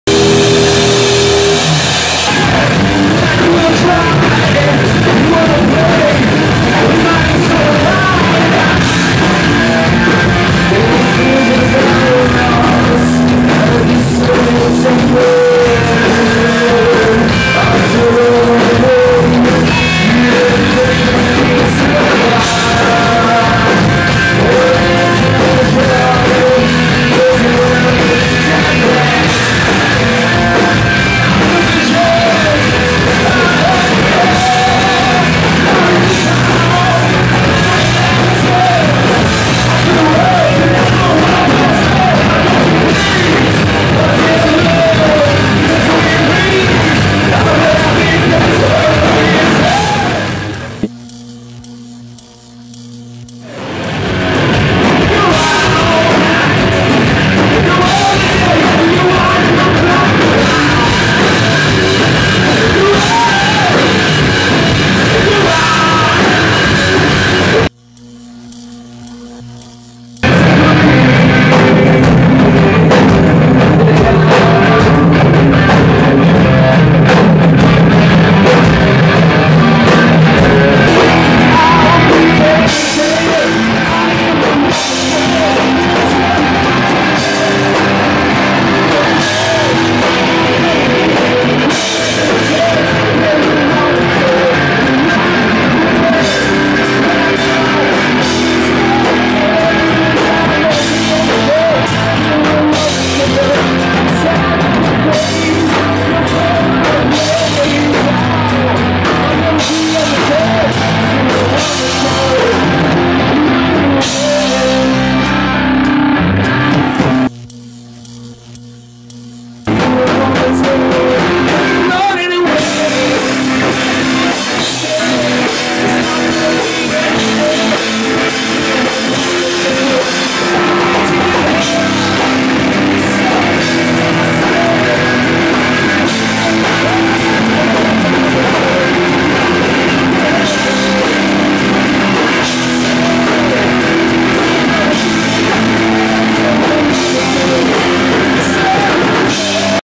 Tinks showreel
A few gigs in france and belgium. tinks on drums